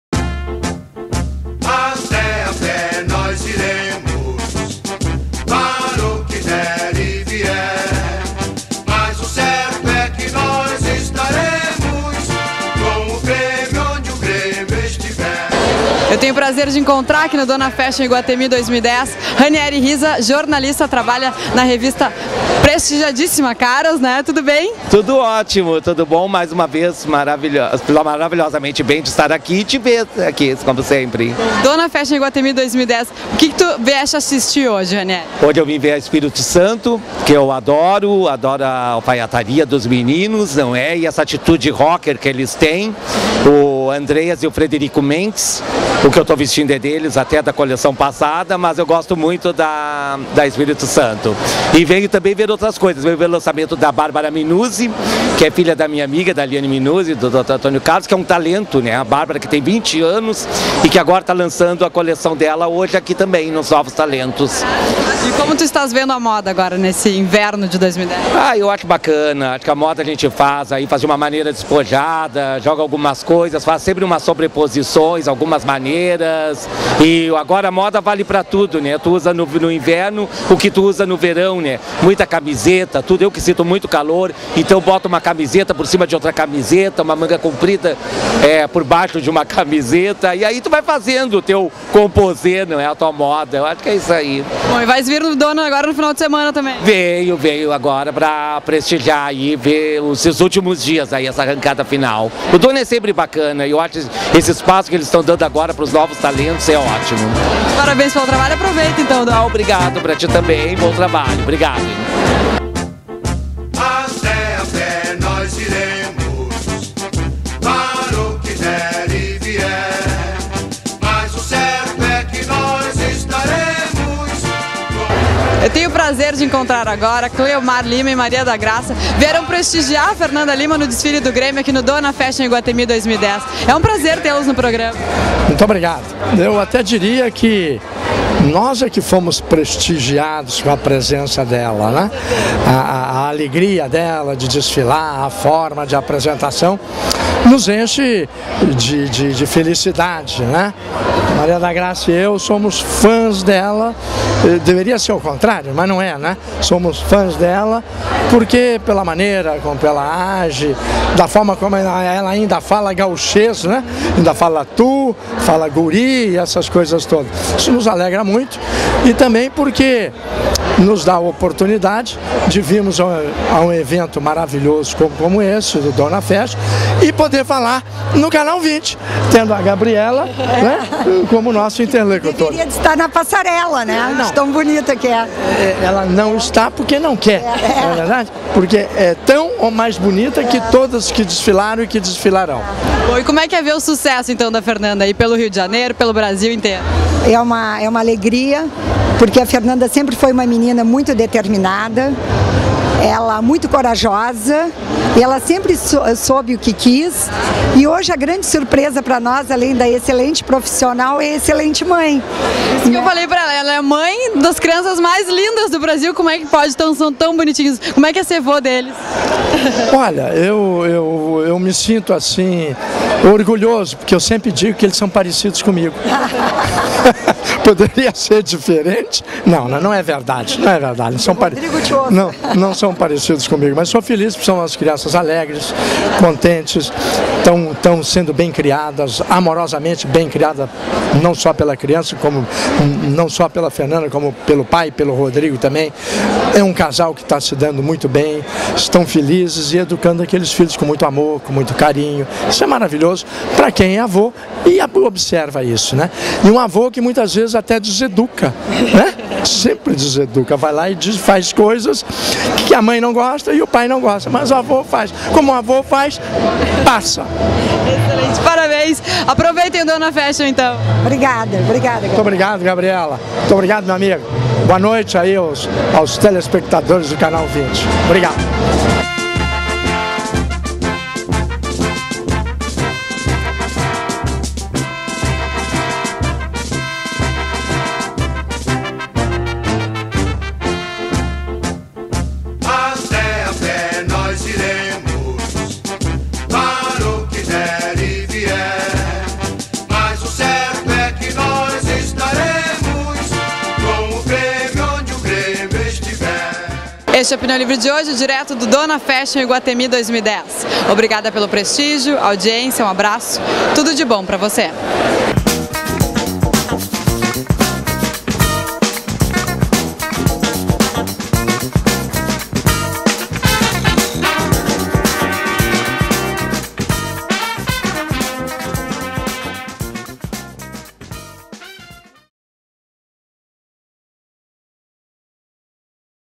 17.04.10 Grêmio FBPA lança coleção 2010 no Donna Fashion Iguatemi Parte 3-3
Gabriella Bordasch  entrevista:
-Ranieri Rizza-jornalista
-Cleomar Lima
-Mara da Graça